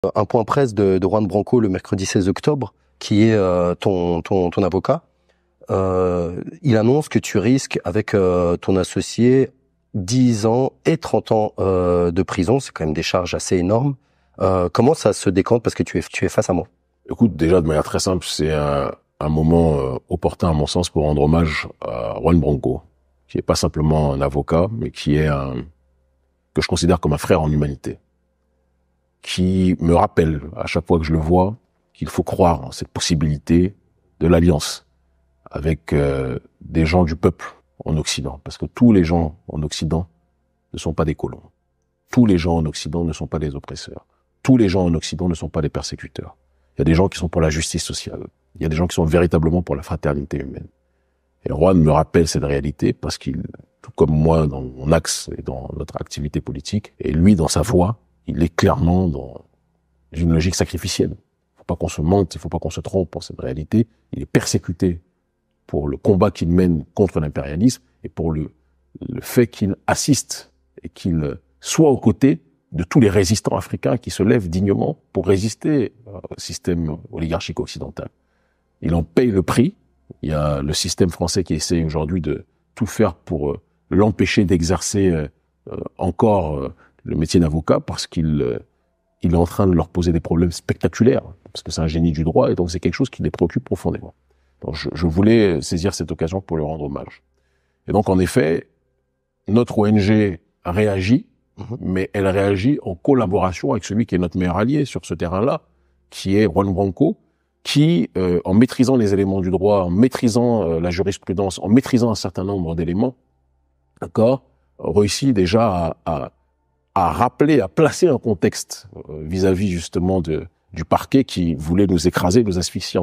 Kemi Seba, arrêté par la DGSI, encourait 30 ans de prison.
Kemi Seba décrit le rôle de l'avocat Juan Branco dans son affaire